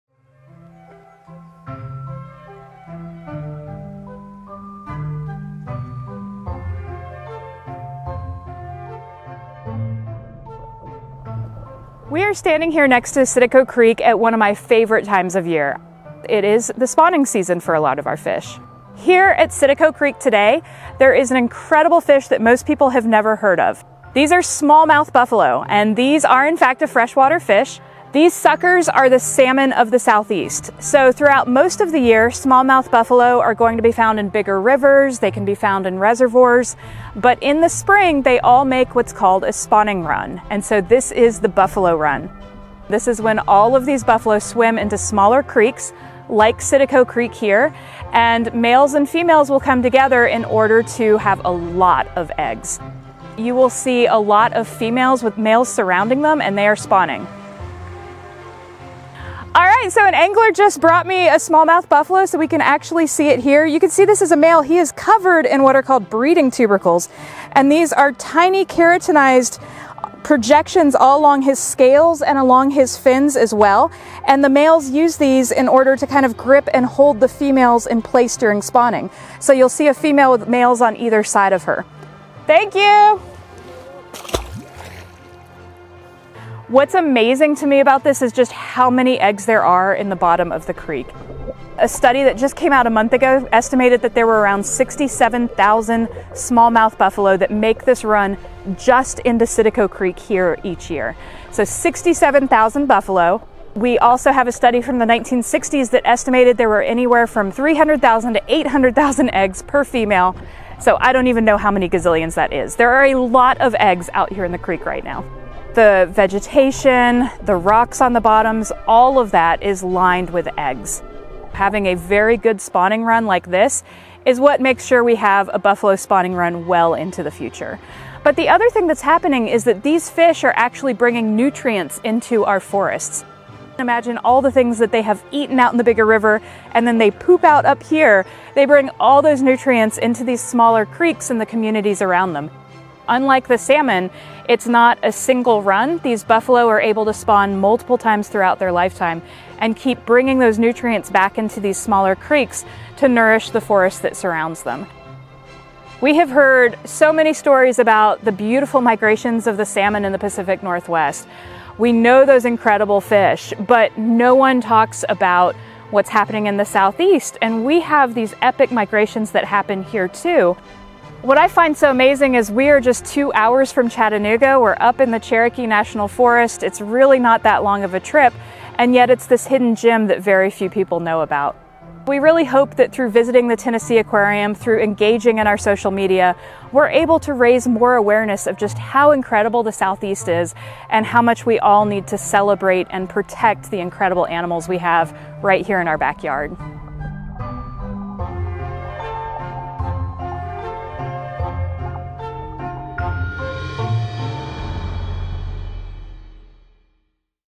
Behold the Small-mouth Buffalo Migration, the Southeast's Answer to the Salmon Run!
Sure, the Pacific Northwest is well-known for the spectacle of its salmon runs, but did you realize the Southeast is host to its own enormous fish migrations? Every spring, tens of thousands of Small-mouth Buffalo push into small streams like Tennessee's Citico Creek to spawn billions of eggs.

Seeing these enormous fish crowding the water to maximum capacity is a sight to behold, one of which relatively few Southerners are even aware!